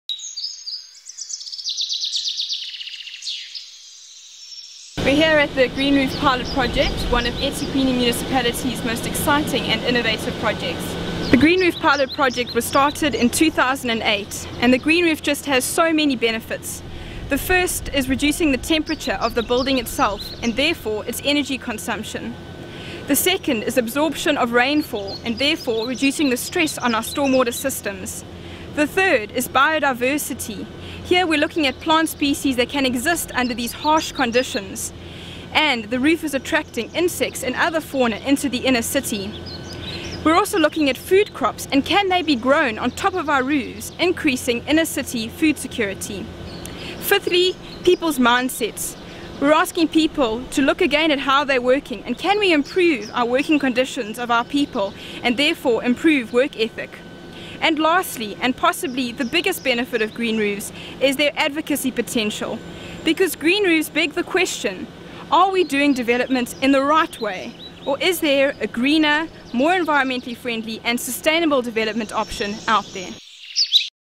Durban - Green Roof
This trial Green Roof project was initiated by the eThekwini Municipality in Durban, South Africa. So far the project has been a tremendous success!